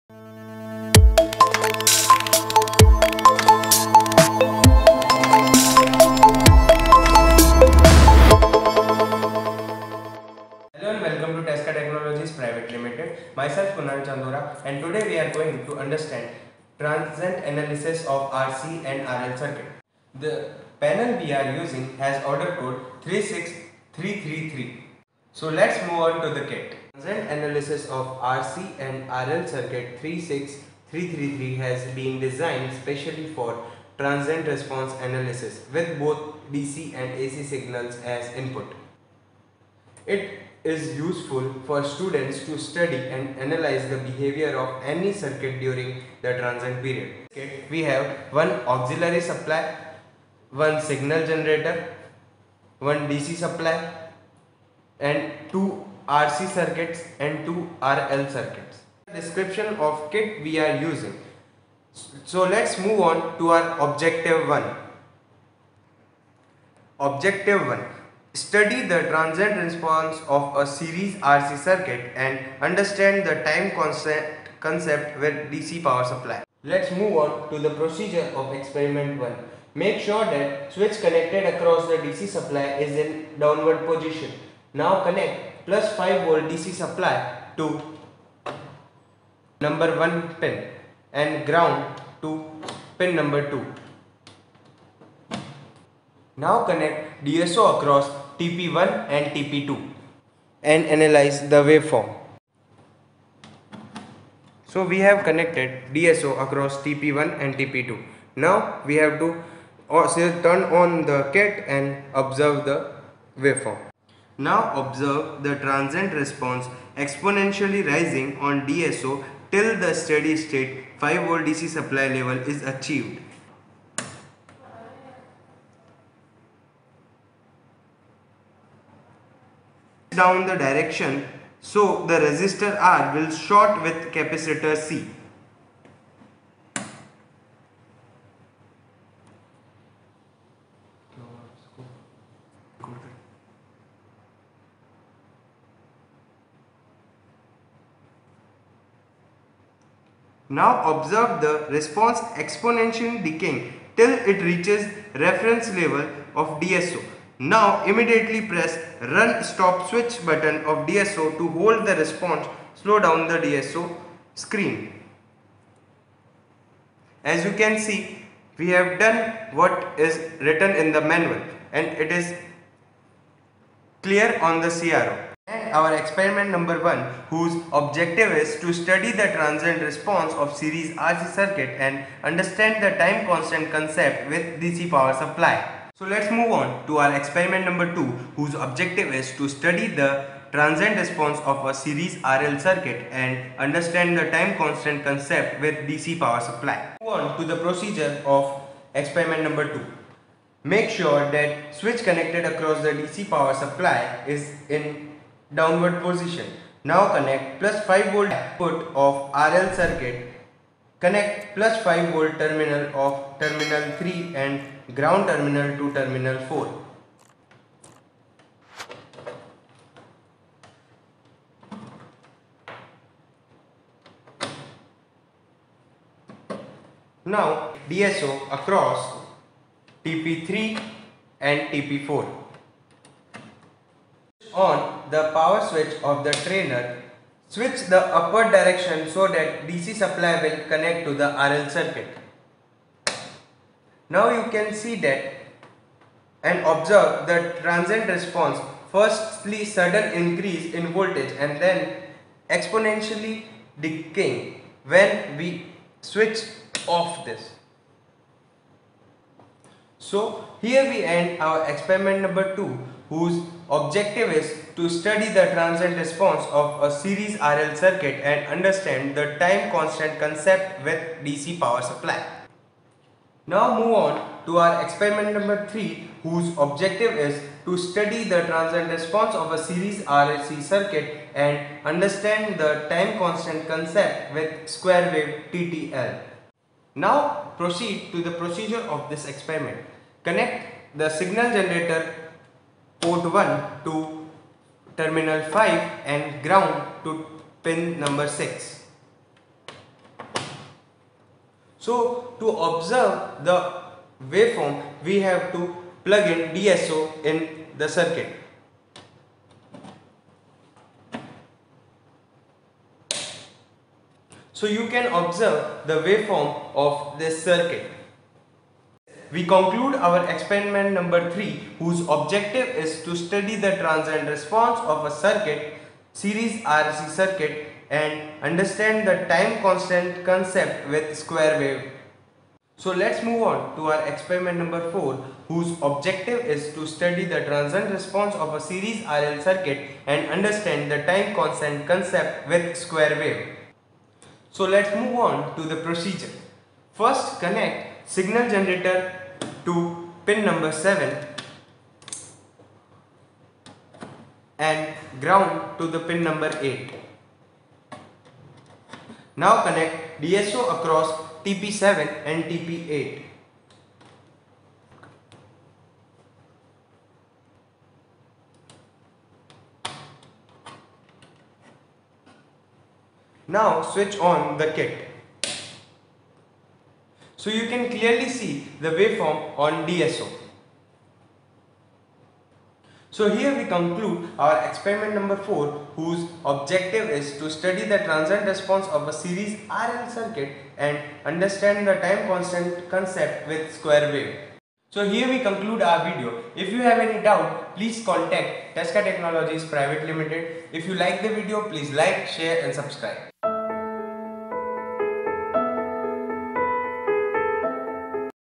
Transient Analysis | First order RC and RL Circuit || Tesca 36333
In this video, we described the Transient Analysis of RC/RL Circuit - Tesca 36333